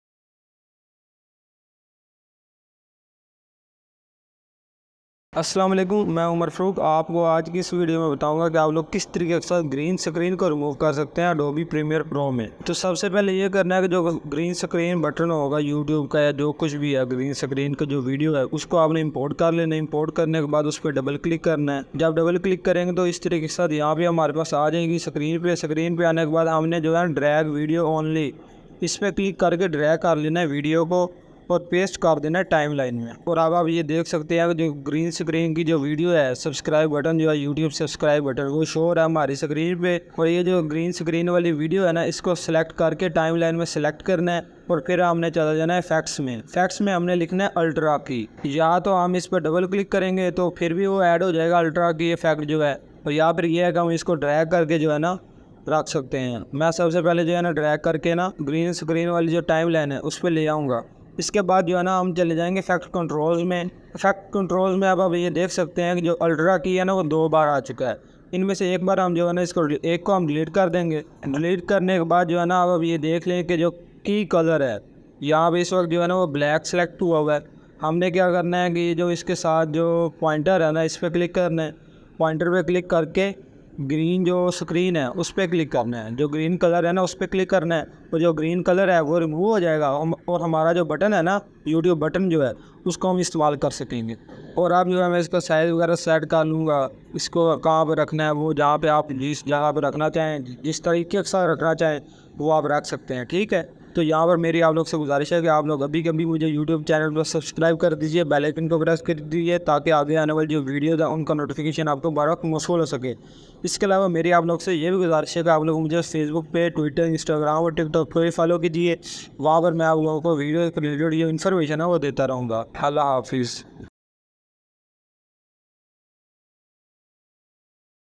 How to Remove Green Screen in Premiere Pro 2023 IN SECONDS
This video provides a comprehensive guide on removing the green screen effect using Adobe Premiere Pro. The steps outlined are incredibly easy to follow and will allow you to remove the green screen from your footage within seconds. Furthermore, these steps are universally applicable across all versions of Adobe Premiere Pro. By implementing this tutorial, you can elevate the quality of your videos to new heights.

_____________________________/SOCIAL LINKS\___________________________________